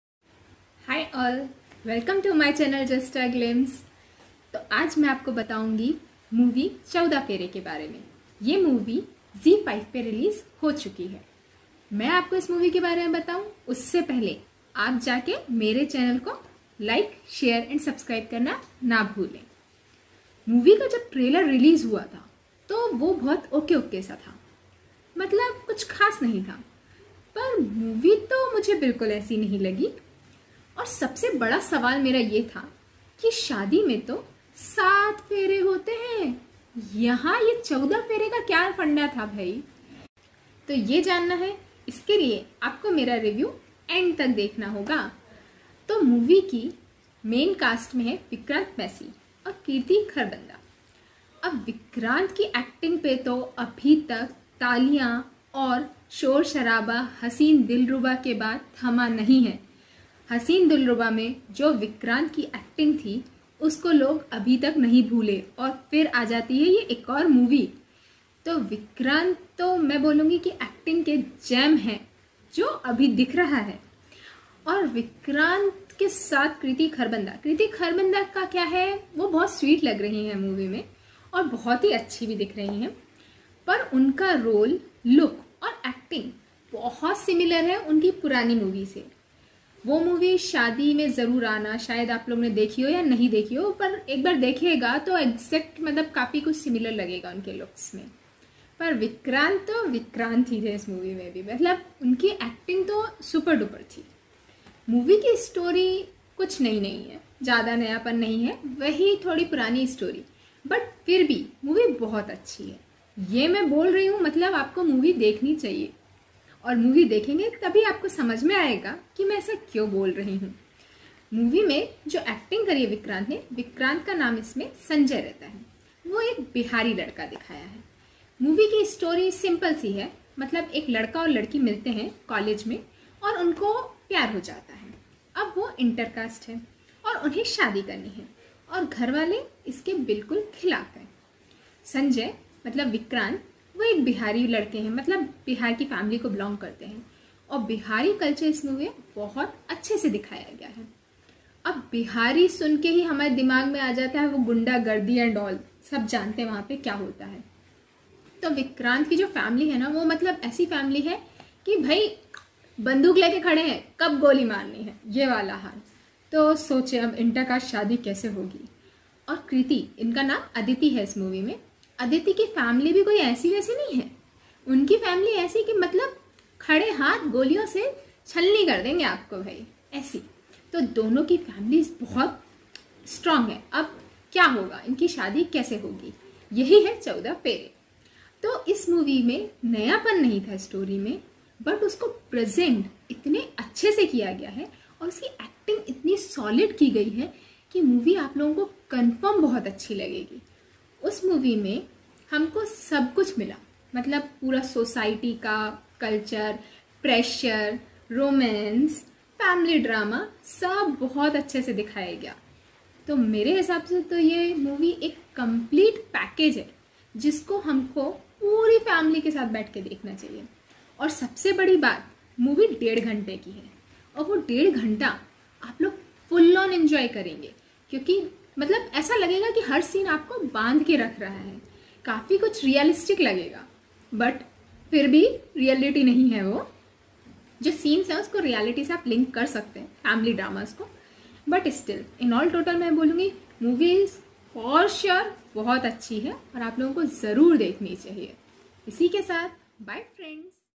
14 phere movie review@Anika
14 phere movie is released on Zee5.
Vikrant massey & Kriti kharbanda is in main cast.
Vikrant acts as Sanjay & Kriti acts as Aditi. Sanjay & Aditi falls in love. Boy belongs to typical bihari family. Wants to marry inter caste. Families are completely against it so how 7 phere turns into 14 phere.